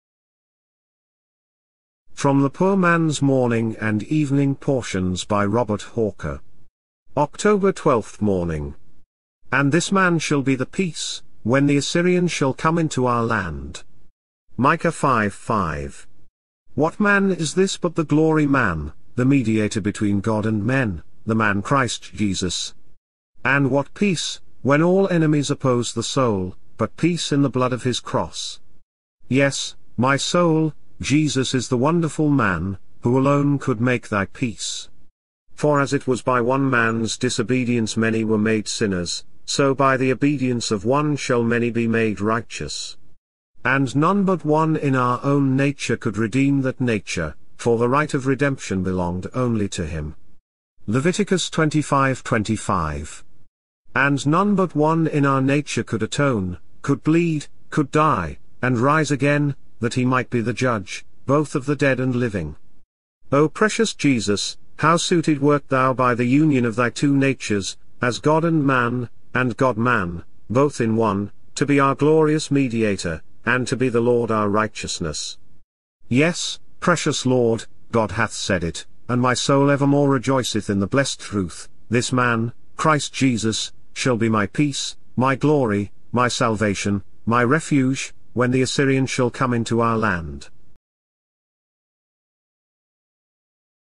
And this man shall be the peace- Micha 5:5- October 12th morning portion
From the POOR MAN'S MORNING AND EVENING PORTIONS by Robert Hawker.  october 12th morning.
"And this man shall be the peace, when the Assyrian shall come into our land."Micah 5:5.  What man is this but the Glory-man, the Mediator between God and men, the man Christ Jesus? And what peace, when all enemies oppose the soul, but peace in the blood of his cross? Yes, my soul, Jesus is the wonderful man, who alone could make thy peace. "For as it was by one man's disobedience many were made sinners; so by the obedience of one shall many be made righteous." And none but one in our own nature could redeem that nature; for the right of redemption belonged only to him. Leviticus 25:25. And none but one in our nature could atone, could bleed, could die, and rise again, that he might be the Judge, both of the dead and living. Oh precious Jesus, how suited wert thou by the union of thy two natures, as God and man, and God-man, both in one, to be our glorious Mediator, and to be the Lord our Righteousness! Yes, precious Lord, God hath said it, and my soul evermore rejoiceth in the blessed truth: this man, Christ Jesus, shall be my peace, my glory, my salvation, my refuge, when the Assyrian shall come into our land.